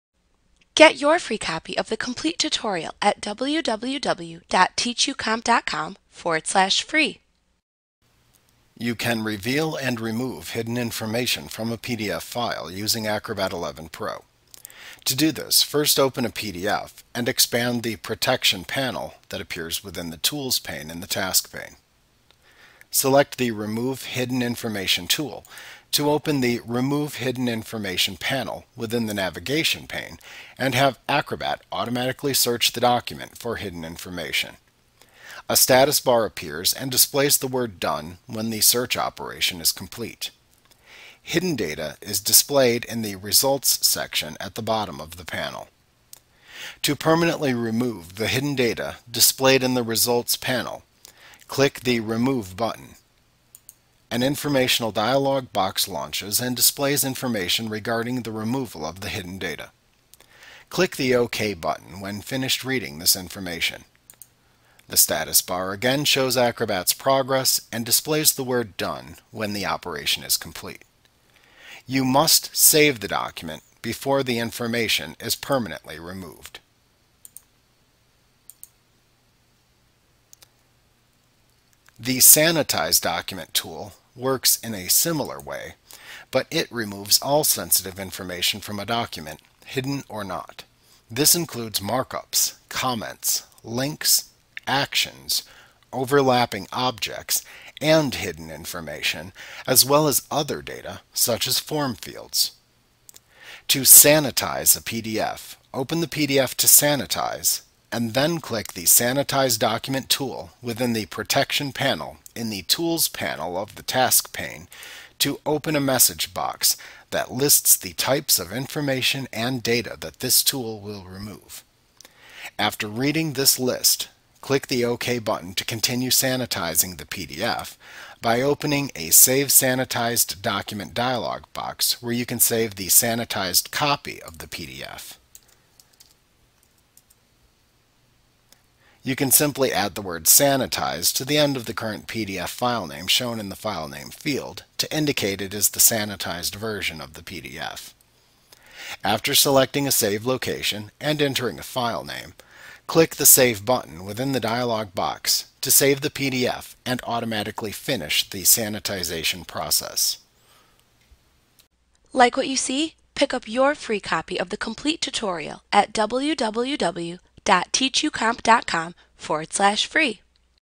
Revealing and Clearing Hidden Information - Adobe Acrobat XI Training Tutorial Course
Learn about Revealing and Clearing Hidden Information in Adobe Acrobat XI Visit us today!